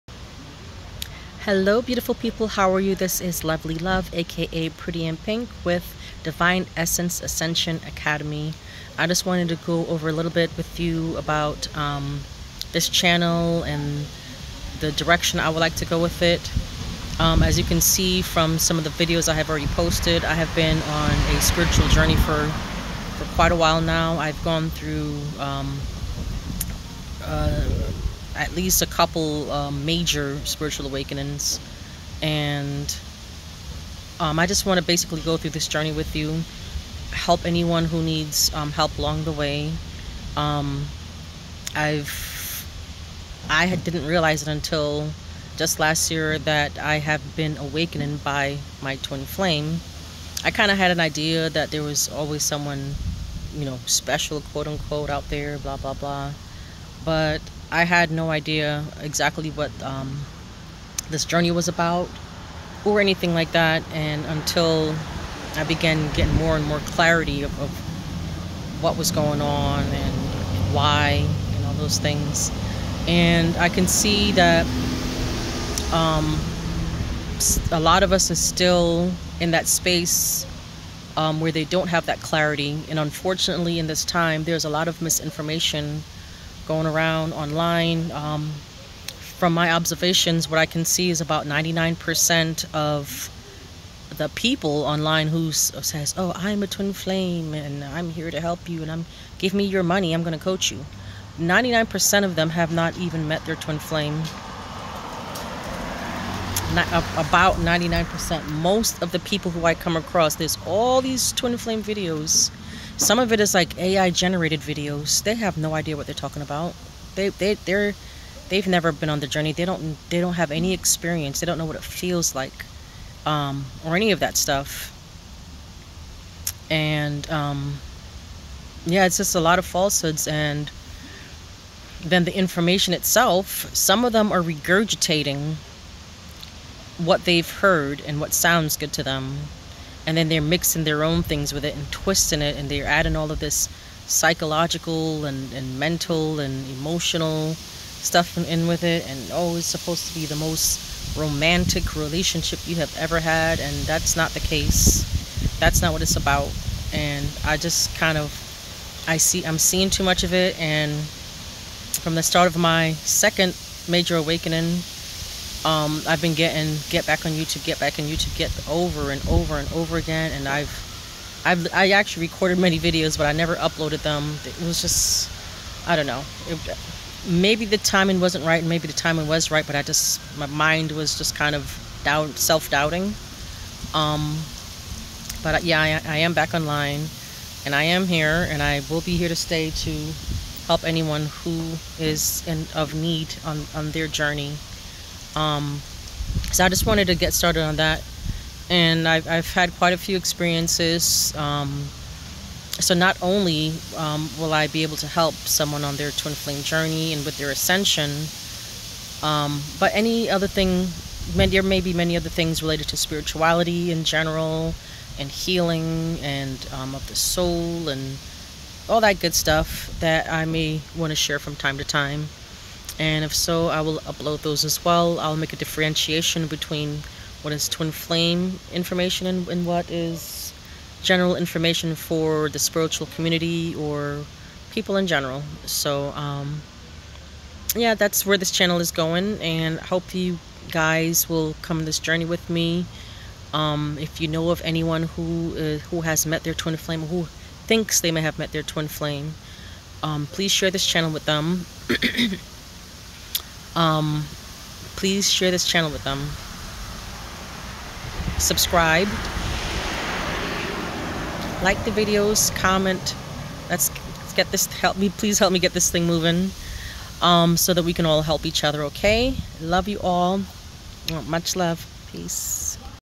TWIN FLAME Coach and Experience 🔥 Introduction to priddyihnpinc and Divine Essence Ascension Academy
I will be assisting Divine Feminine, Spiritual Twins, to maneuver through your Twin Flame Spiritual Journey and Ascension.

In this video I provide a simple introduction as a Coach for Divine Feminine Twins and Spiritual Twins on a Twin Flame Journey.

Let me help guide you towards Union and Unity Consciousness.

We will only be accepting a few members at this time for Twin Flame Coaching and support, at no charge.